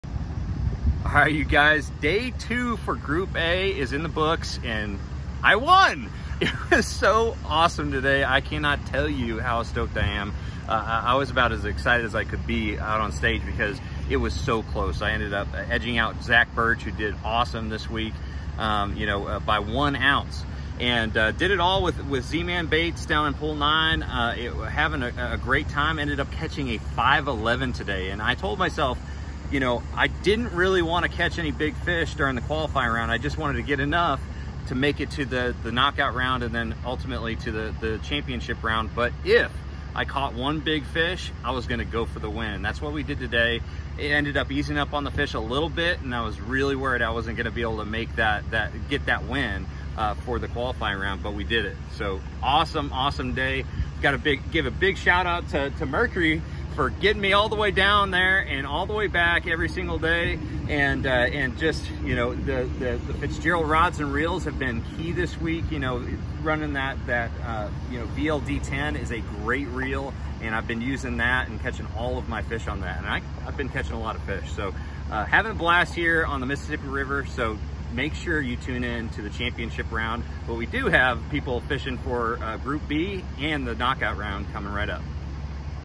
“Sonar” Wins Group A
Mikes “Sonar” Burghoff won today’ Elimination Round at the MLF BIG 5 Title on the Mississippi River and will advance straight to Sunday’s Championship Round!